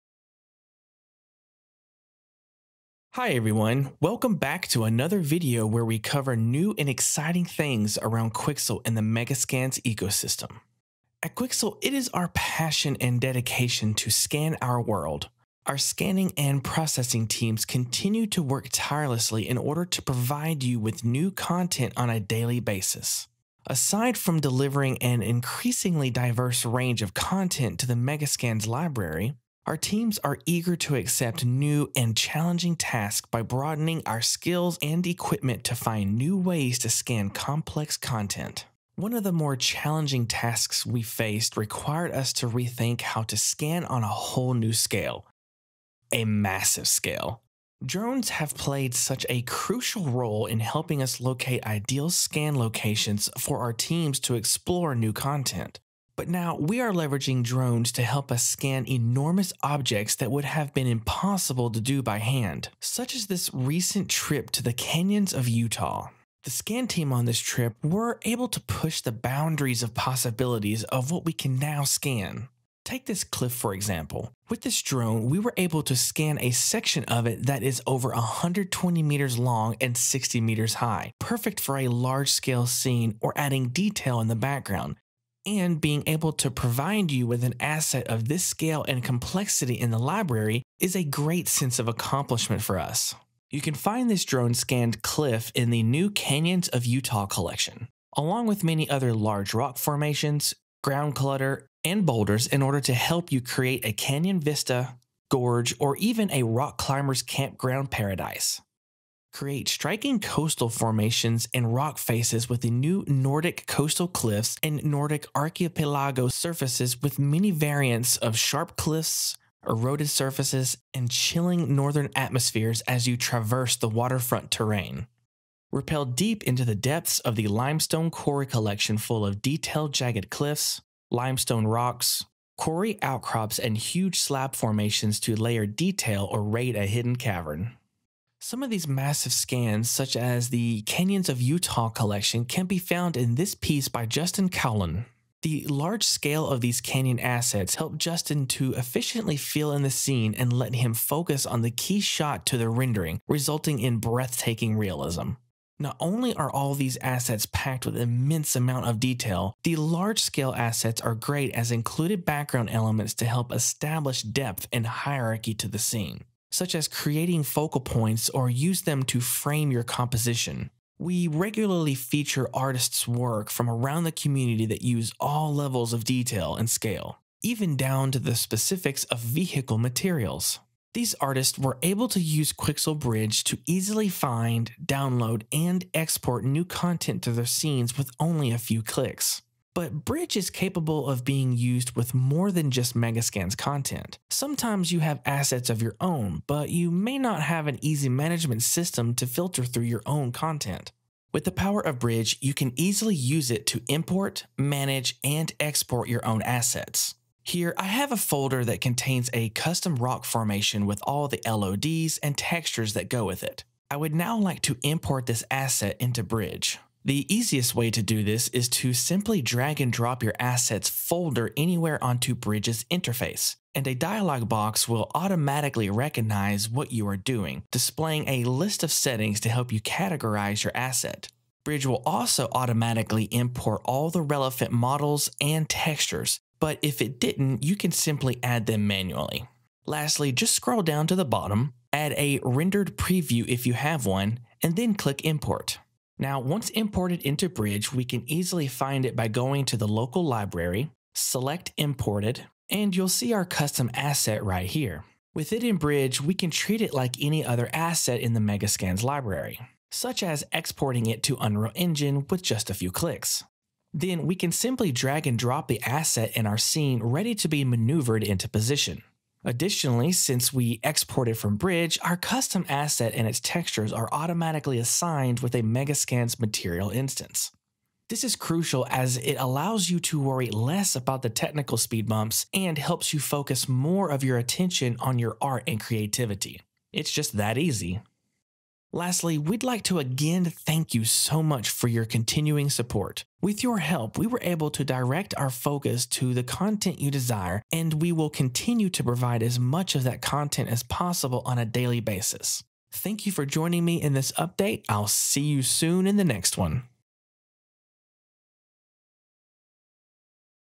메가스캔과 브리지 2
드론을 활용한 대규모 스캔 방법을 살펴보시고, Canyons of Utah 및 Nordic Coastal Cliffs 등 메가스캔 라이브러리의 신규 컬렉션을 만나보세요.

*언리얼 엔진 5에서는 브리지가 통합되었습니다.